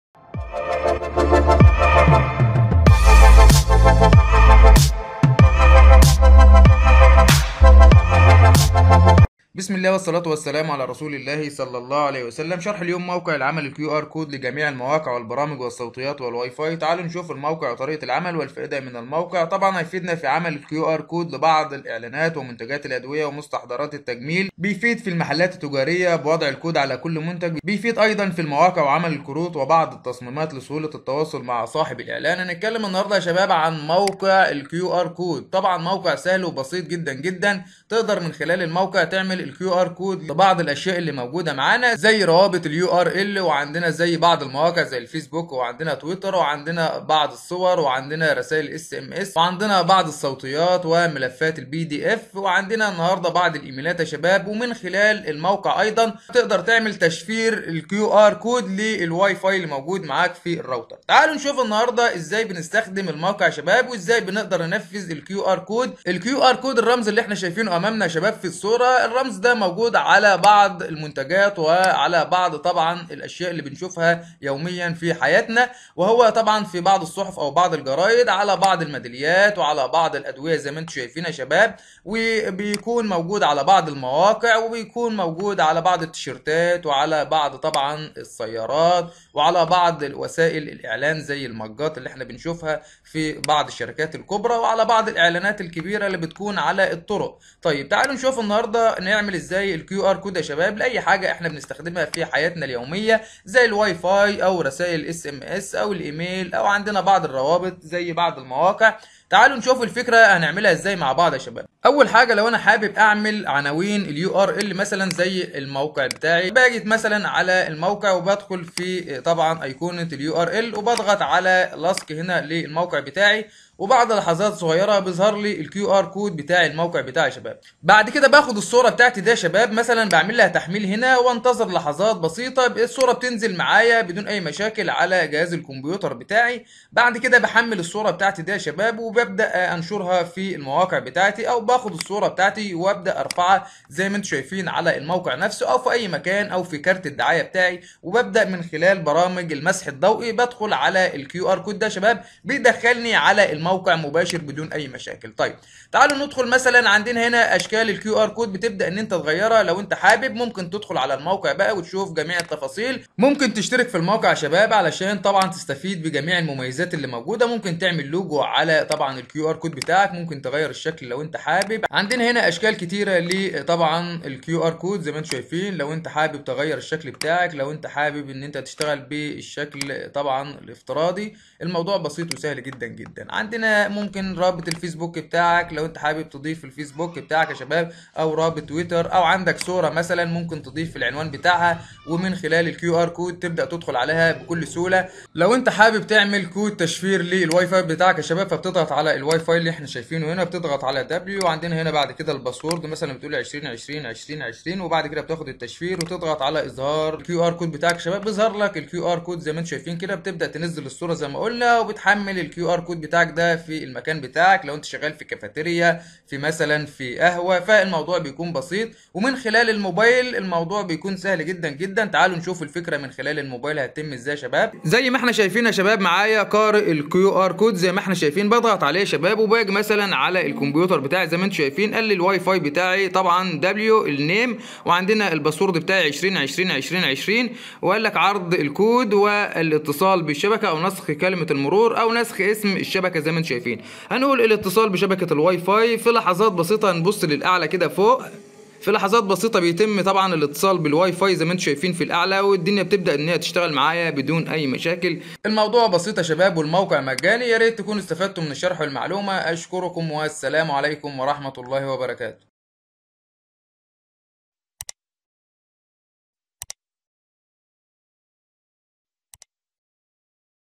موقع مجانى لعمل الباركود احمد العربى
موقع مجانى لعمل QR كود للجميع المواقع والايميلات والتطبيقات وتشفيرالواى فاى